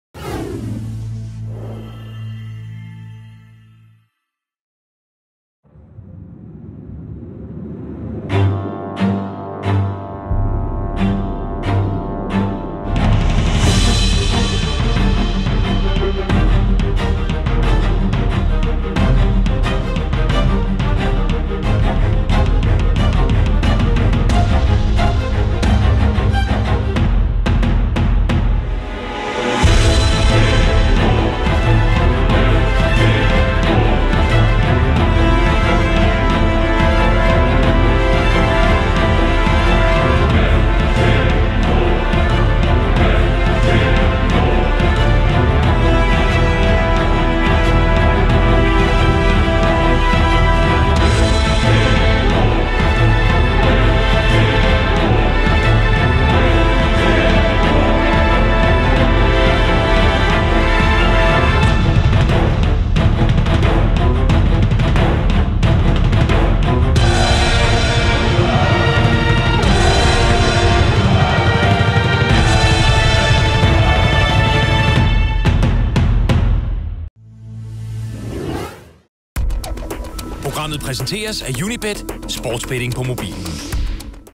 Se startopstillingen mod FC Dnipro
Manager Ståle Solbakken har nu valgt de elleve spillere, der skal starte i den vigtige kamp mod ukrainske FC Dnipro. Se spillerne præsenteret i videoen her:

Andersen, Høgli, Nilsson, Jørgensen, Bengtsson, Amankwaa, Amartey, Delaney, Jørgensen, Kadrii og Cornelius.

Bænk: Wiland, Antonsson, Claudemir, Mussis, Toutouh, De Ridder og Pourié.